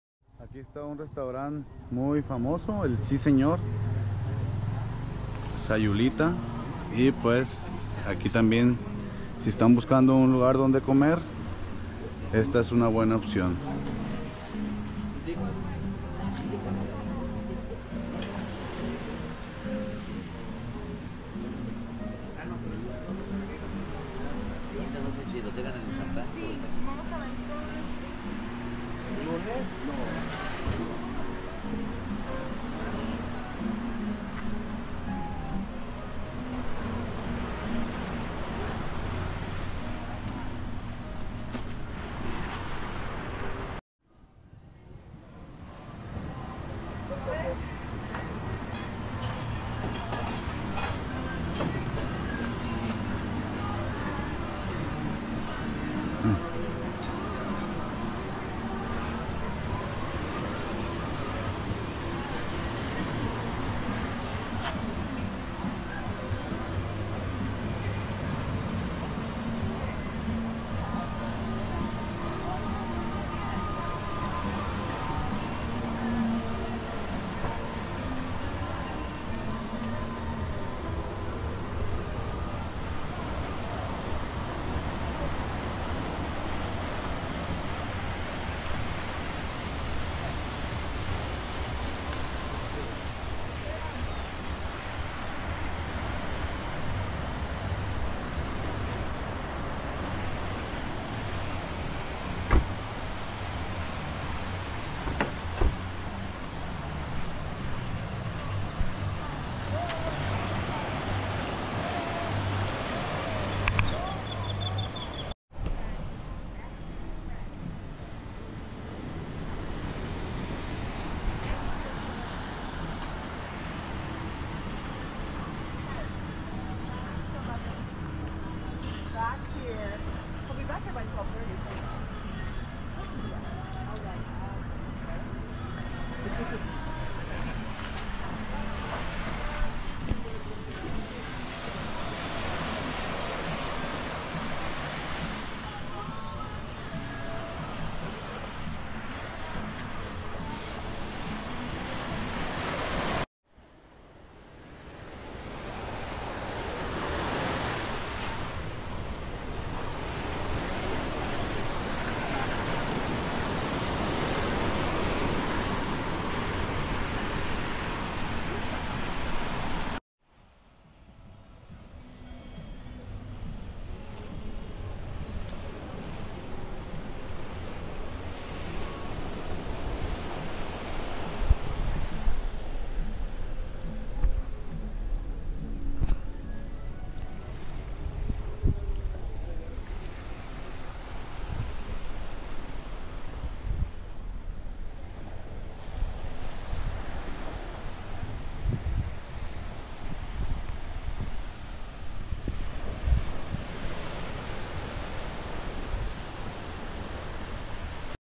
Restaurante Si Señor en la mejor zona de Sayulita 🍽️🌴
Te mostramos cómo es el restaurante Si señor en sayulita nayarit mexico 🍽️🌴jopamworld